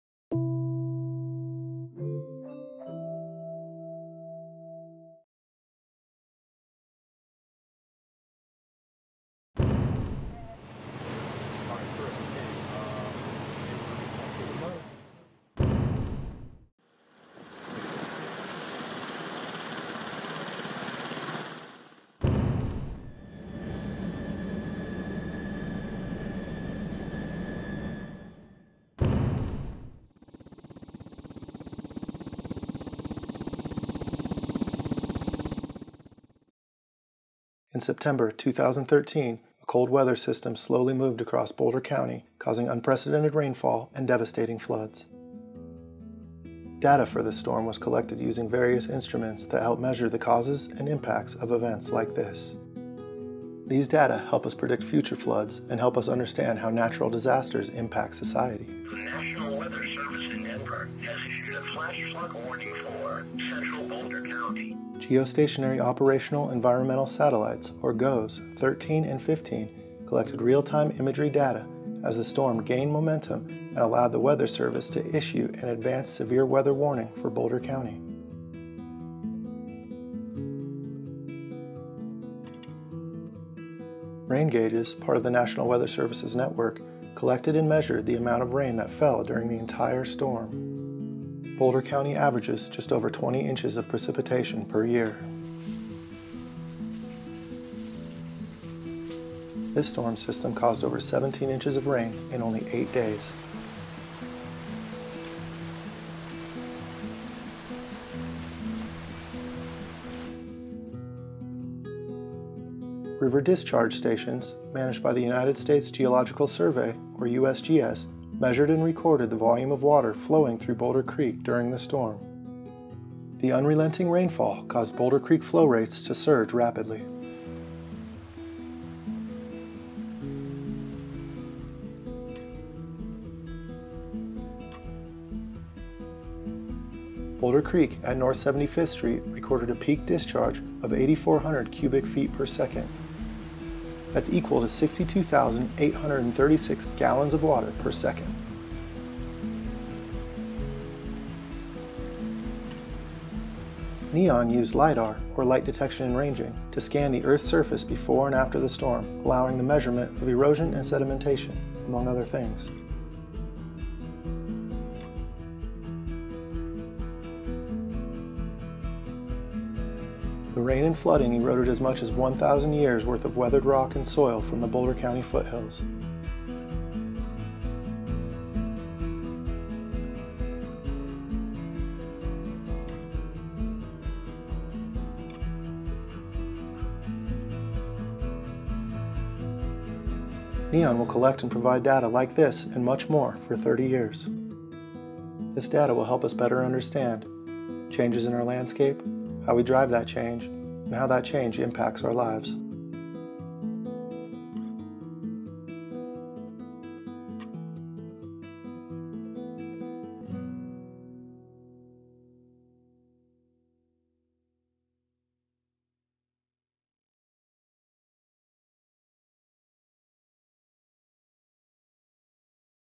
Using Data to Understand the Causes & Impacts of the 2013 Boulder Floods
Data can help us understand the causes and effects of natural disturbance events.  This video delves into the data that allow us to better understand the September 2013 flooding events along the Front Range of Colorado.  We track the storm through satellite imagery, rainfall and stream gauge data and then look at the consequences of the storm through LiDAR measurements and satellite imagery. The National Ecological Observatory Network (NEON) project will collect data like this for over 30 years to help monitor changes in our environment.

Suggested citation for this video: National Ecological Observatory Network. 2016 November 7. Measuring the Causes and Impacts of the 2013 Floods [Video file]. Courtesy: Battelle.